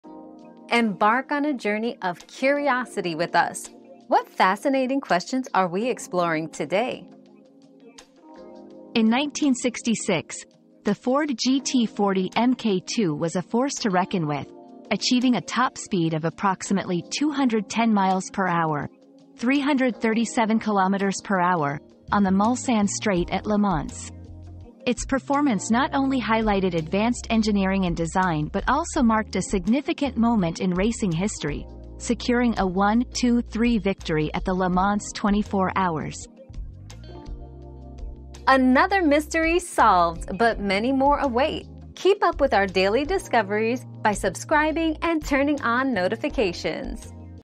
How fast was the GT40 in 1966?
Unleashing the GT40: The 1966 Speed Demon 👉 Speed Demon GT40 👉 Discover the incredible top speed of approximately 210 mph achieved by the Ford GT40 Mk II in 1966 at Le Mans, showcasing its groundbreaking engineering and historic racing triumph.

Laura S. Harris (2024, March 31.) How fast was the GT40 in 1966?
    🌐 AskAbout.video